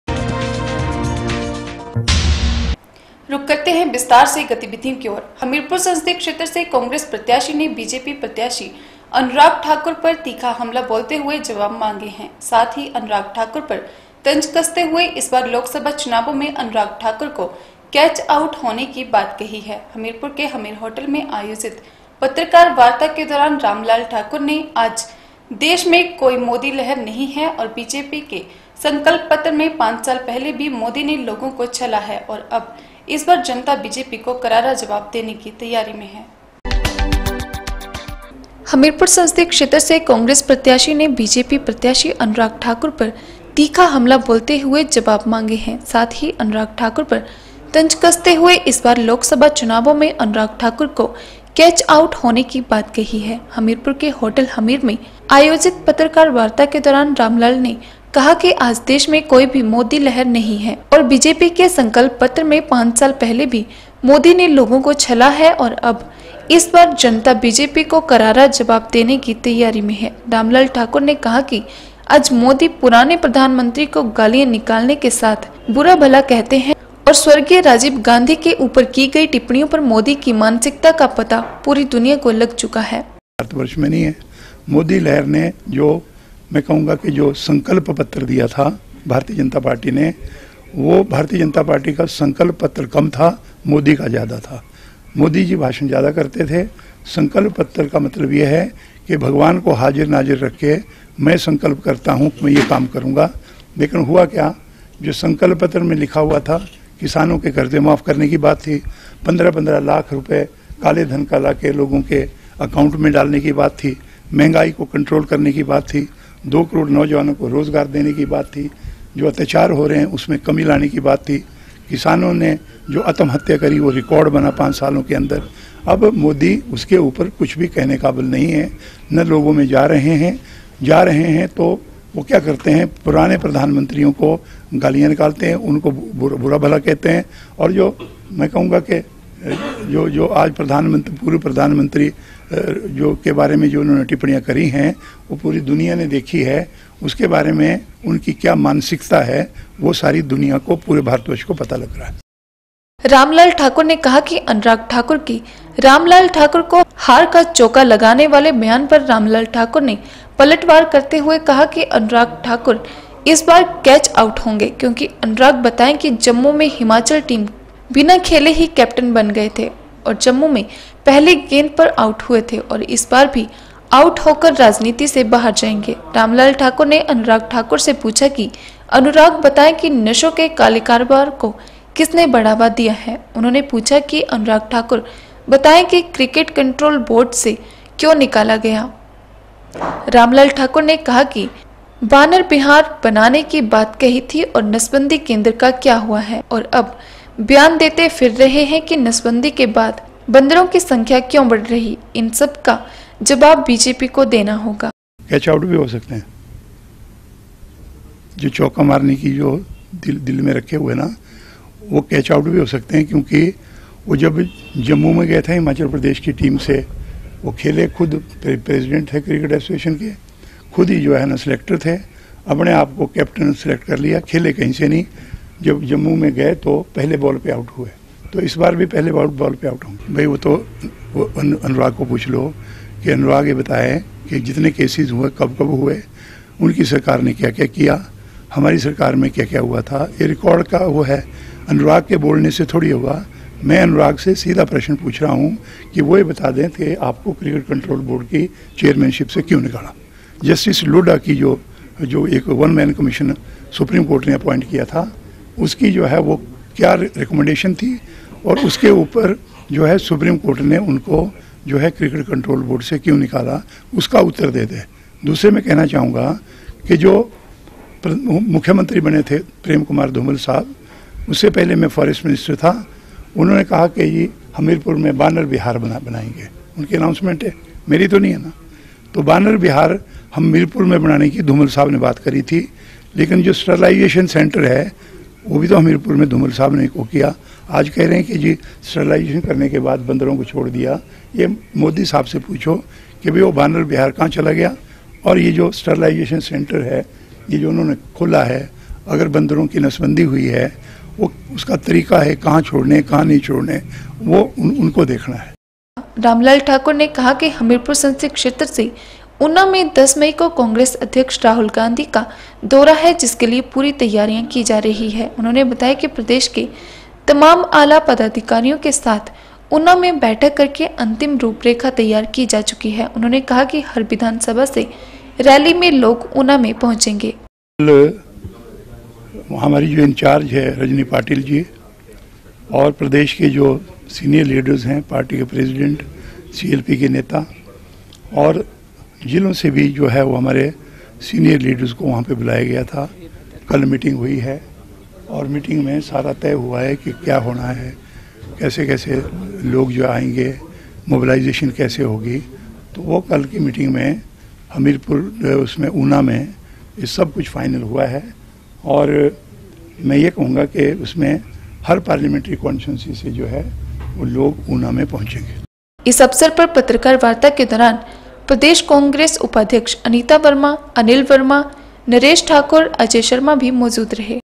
8 MAY N 1 Congress candidate seeks response from BJP candidate Anurag Thakur
हमीरपुर संसदीय क्षेत्र से कांग्रेस प्रत्याशी ने बीजेपी प्रत्याशी अनुराग ठाकुर पर तीखा हमला बोलते हुए जबाव मांगे है। साथ ही अनुराग ठाकुर पर तंज कसते हुए इस बार लोकसभाचुनावों में अनुराग ठाकुर को कैच आउट होने की बात कही है।